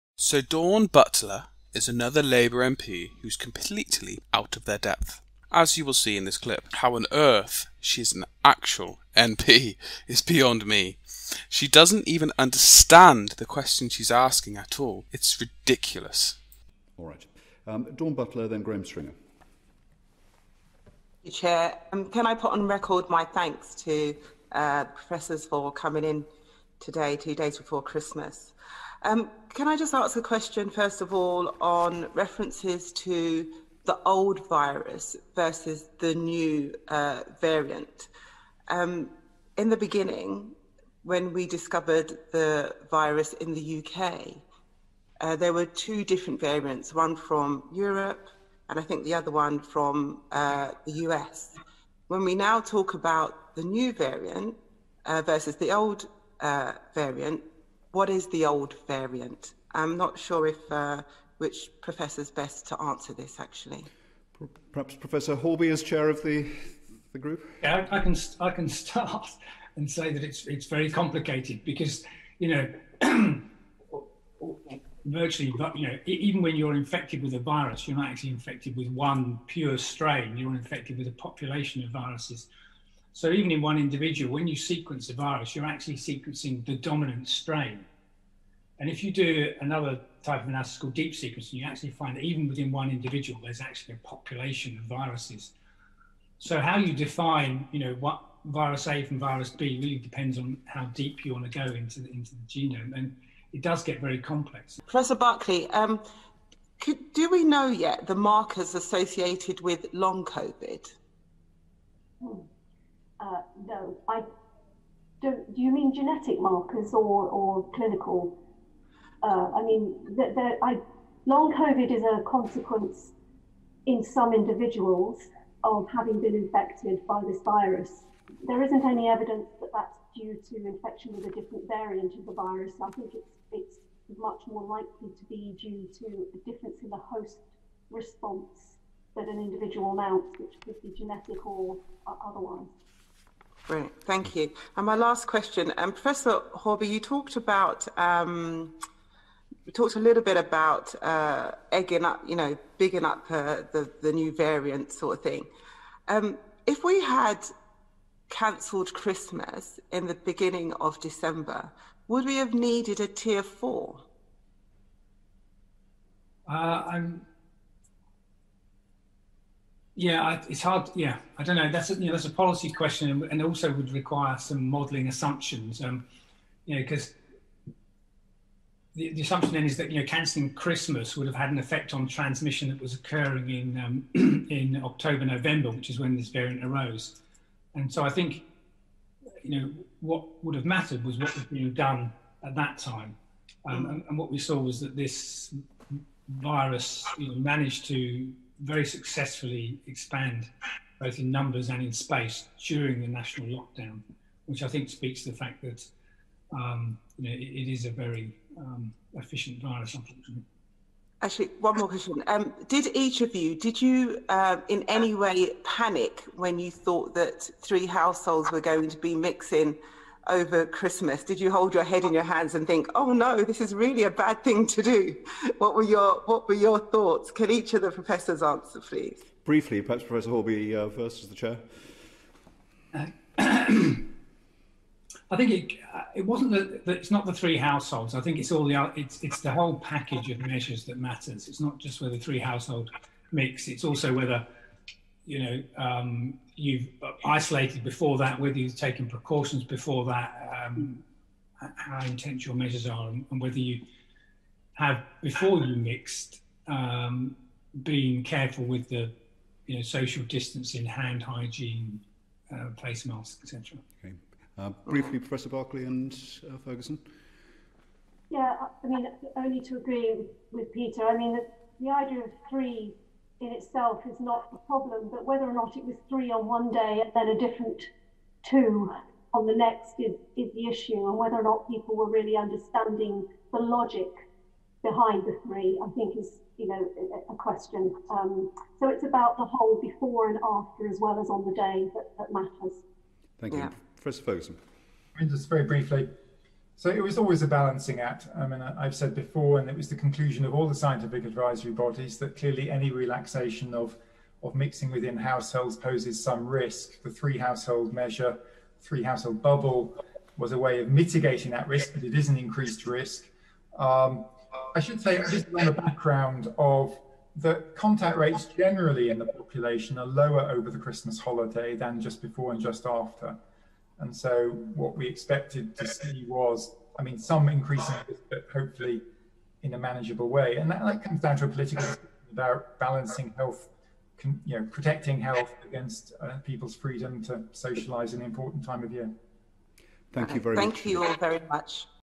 Dawn Butler speaking to Scientists “the new variant sort of thing”
Dunning–Kruger effect here.

Labour 'MP' Dawn Butler laughed gets at by Scientists.

The  Science and Technology Committee try their best to be polite with her don't they.